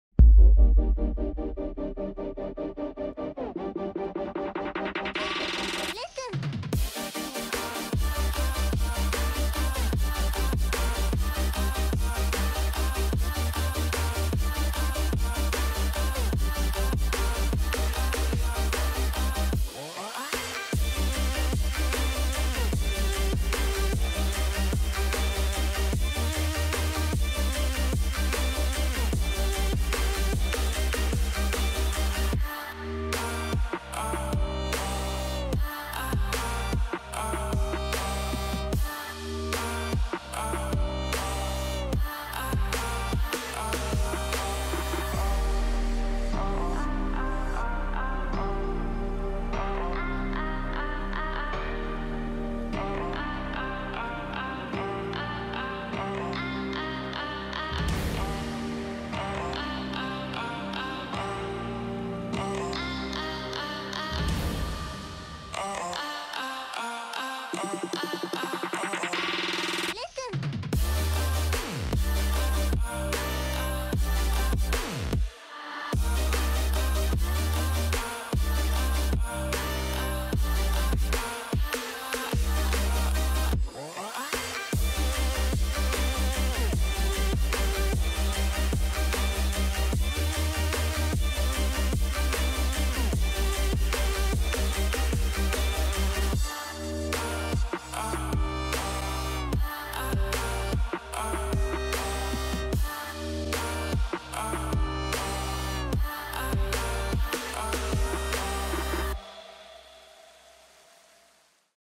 American car show // FHRA 27.3.2016
Kävästiin sunnuntaina messuilla ja sen verran materiaalia tuli kuvailtua niin miksei sitten tekis videota :D
Lähiviikkojen aikana luultavasti pääsee taas ajelemaan niin sitten päästään taas ajovideoitten pariin!